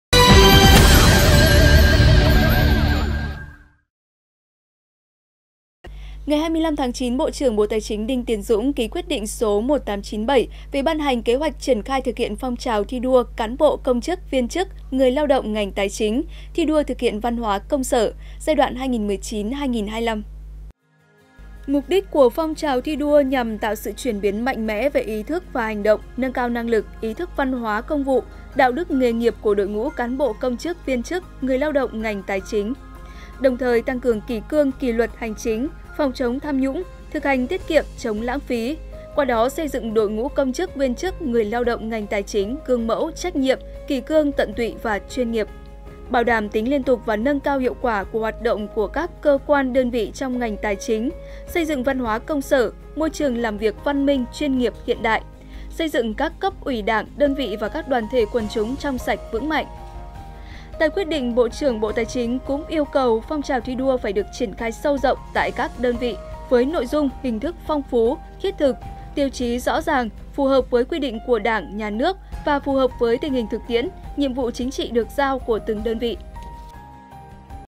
Bộ Tài chính triển khai phong trào thi đua văn hoá công sở
Ngày 25/9, Bộ trưởng Bộ Tài chính Đinh Tiến Dũng ký Quyết định số 1897 về ban hành Kế hoạch triển khai thực hiện Phong trào thi đua “Cán bộ, công chức, viên chức, người lao động ngành Tài chính thi đua thực hiện văn hóa công sở” giai đoạn 2019 – 2025.